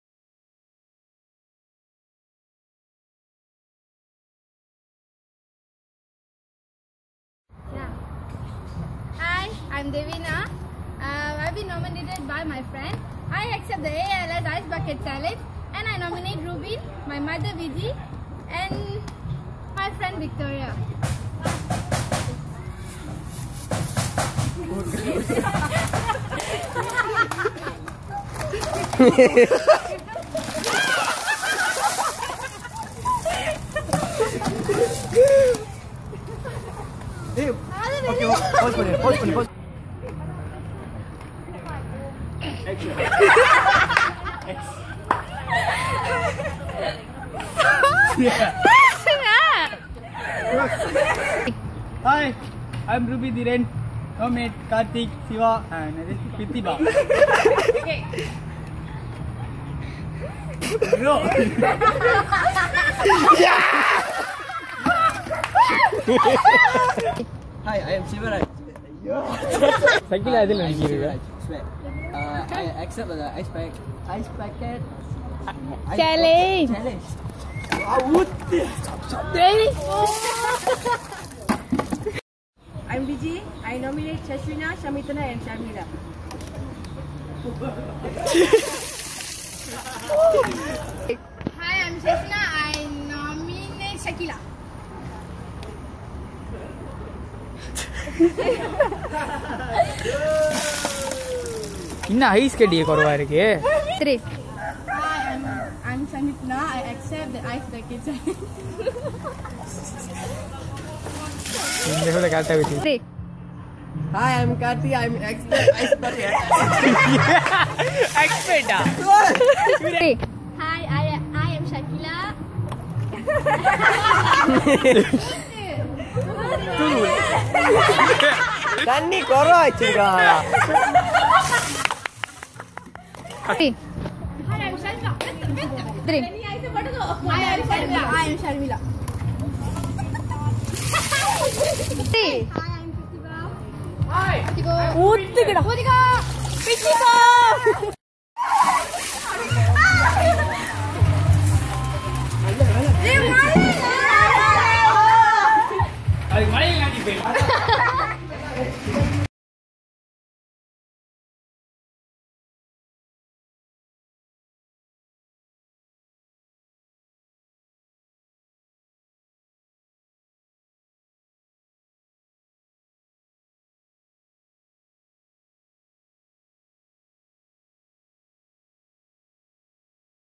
ALS Ice Bucket Challenge by Our Family
We Accept the ALS Ice Bucket Challenge!